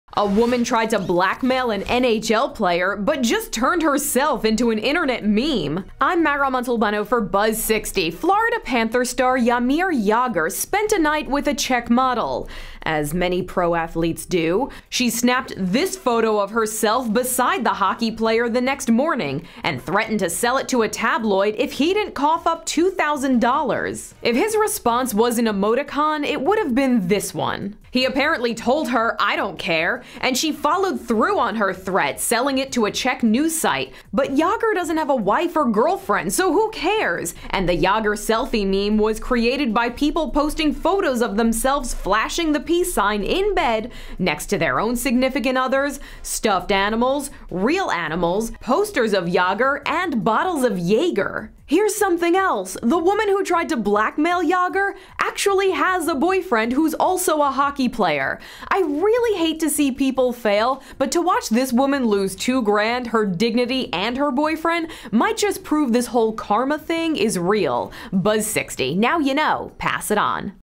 Model attempts to blackmail NHL's Jaromir Jagr & gets memed by Internet
A model tried to blackmail Florida Panthers star Jaromir Jagr, but failed. As Mara Montalbano (@maramontalbano) tells us, the Internet is laughing at her with their own JagrSelfie posts.